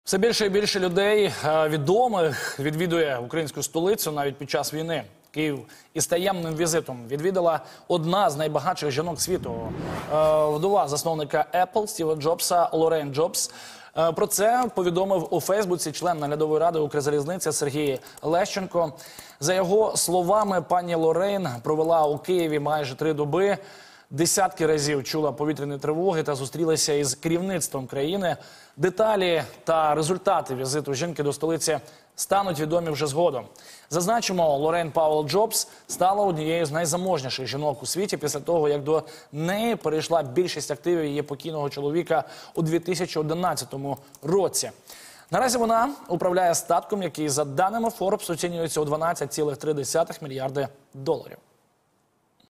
Україну відвідала вдова Стіва Джобса, меценатка Лорен Пауелл Джобс
Київ відвідала одна з найзаможніших жінок світу Лорейн Пауелл Джобс – вдова засновника Apple Стіва Джобса.